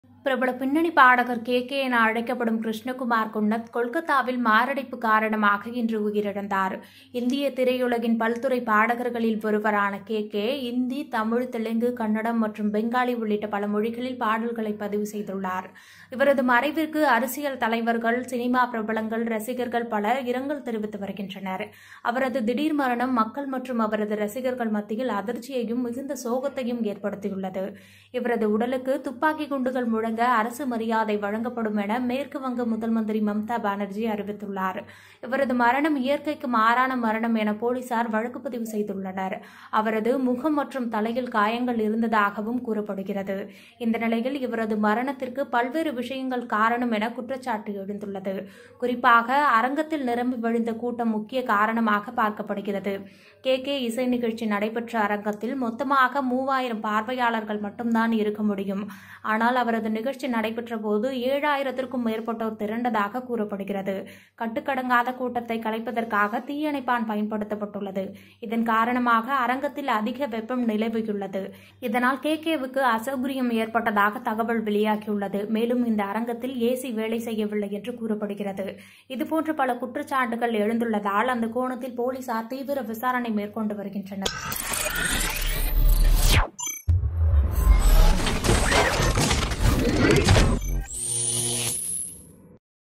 பாடகர் கேகே மரணம்.. விசாரணையில் திடீர் திருப்பம்..! | Krishnakumar Kunnath Death
பாடகர் கேகே மரணம்.. விசாரணையில் திடீர் திருப்பம்..! | K.K.Death